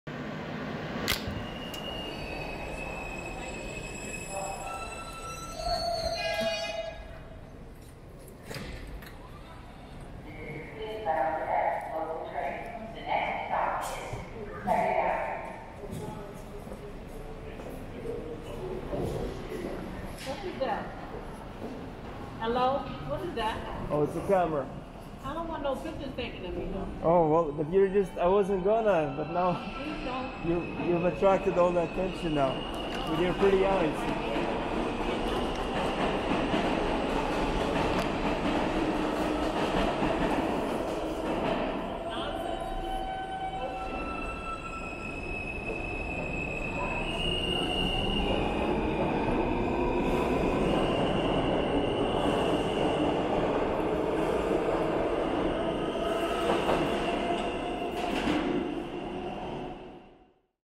Conductor lady to Train Cam: "Nonsense! Bulls--t!"
This F train conductor was probably just having an off day. Clearly not in the mood to have her pictures taken.

Footage taken at Delancey Street Station. February 2022. In 4K